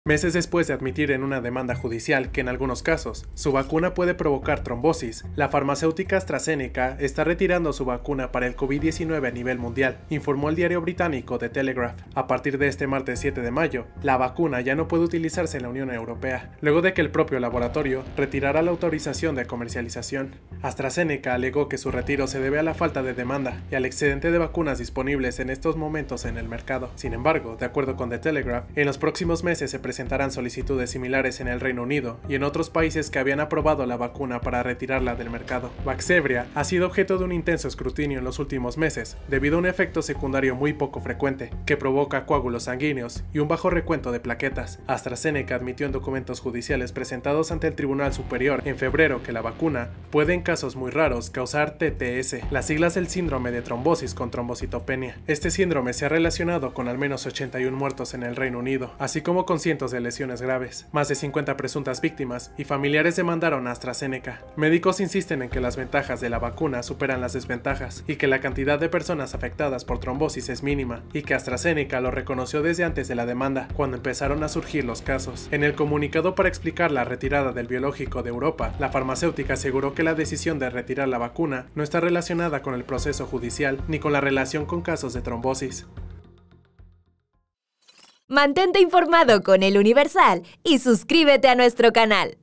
AstraZeneca retira a nivel mundial su vacuna anti-Covid, después de admitir casos de trombosis
El químico ya no podrá utilizarse en la Unión Europea, luego de que el propio laboratorio retirara la autorización de comercialización